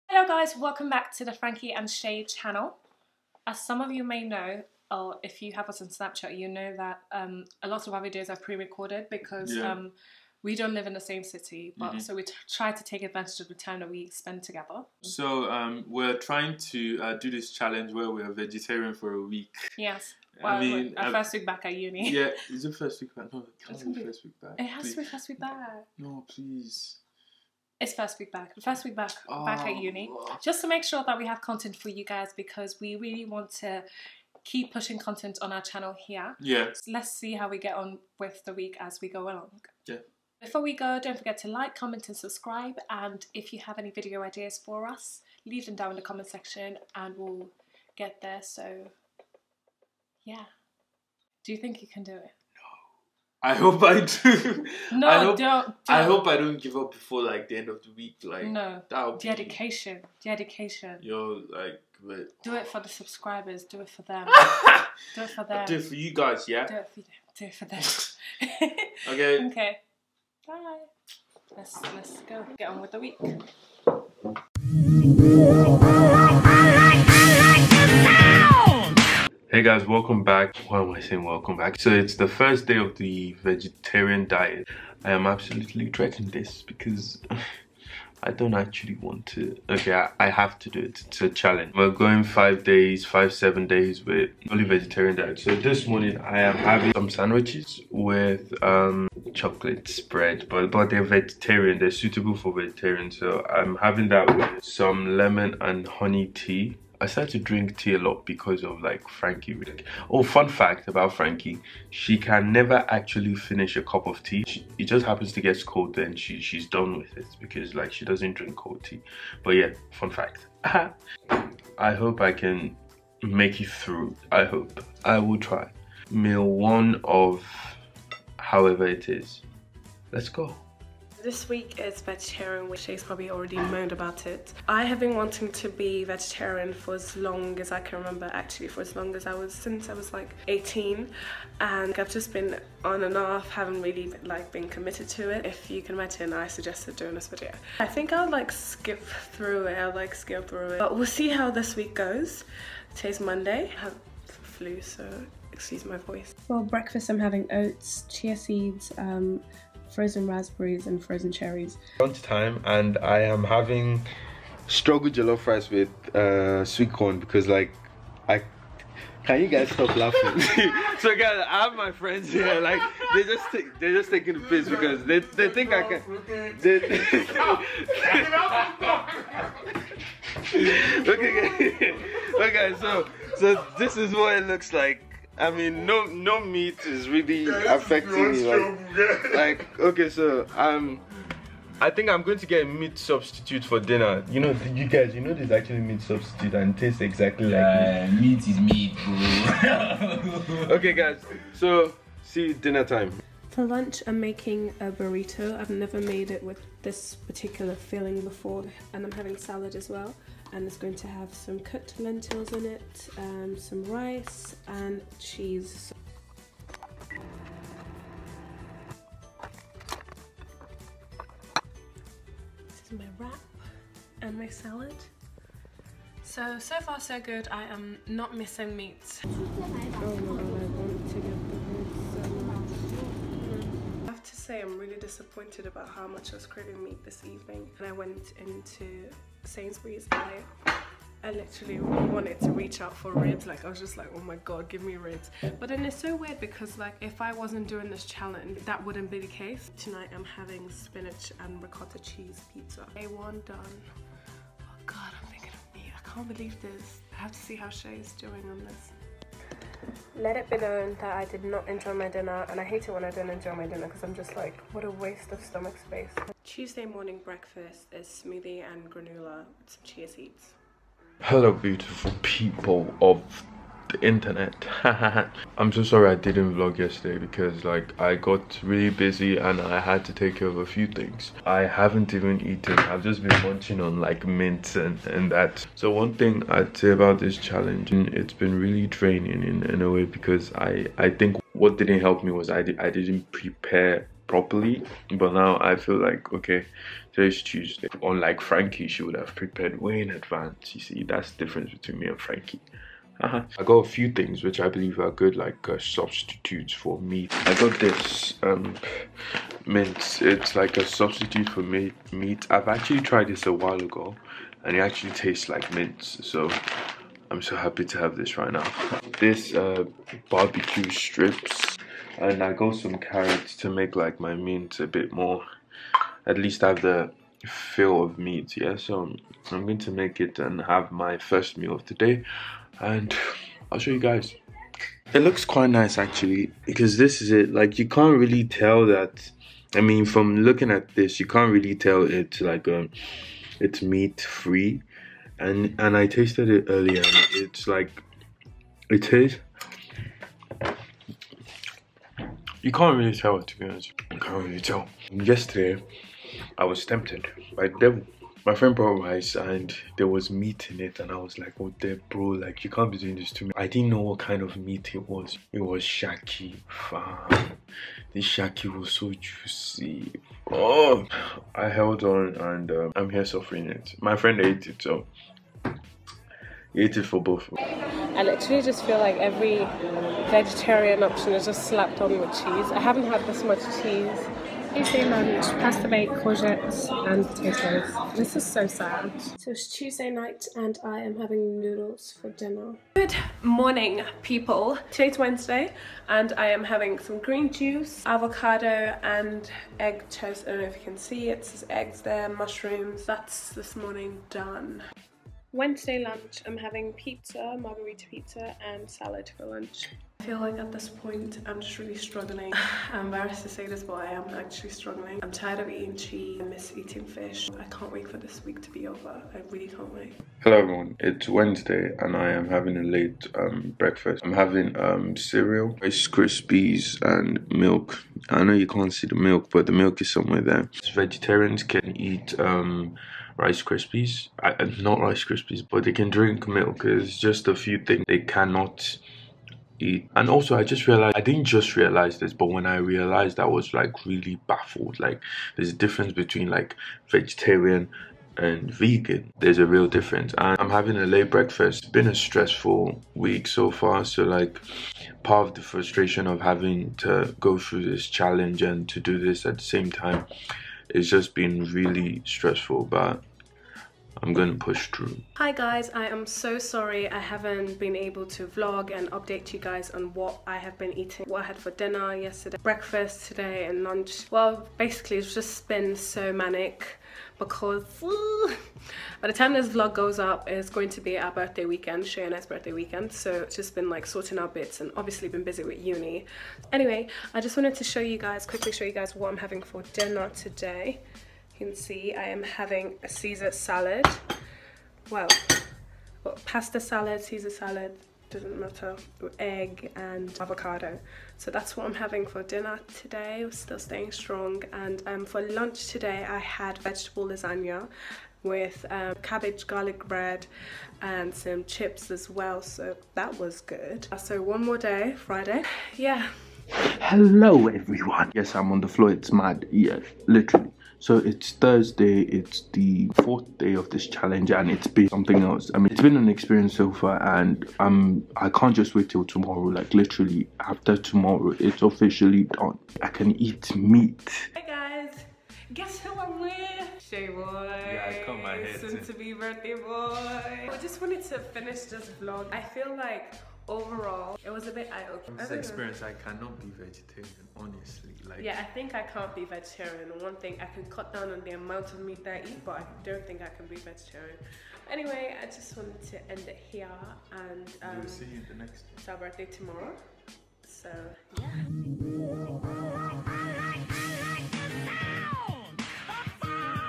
WE TRIED BEING VEGETARIANS FOR A WEEK!
Back at it with another challenge! As meat eaters we tired to take up a challenge and give up meat for a week!

THIS IS HOW WE GOT ON!

*This video was meant to go up yesterday (Friday)! apologies for the delay

DO YOU HAVE ANY VIDEO IDEAS FOR US? LEAVE THEM IN THE COMMENTS SECTION!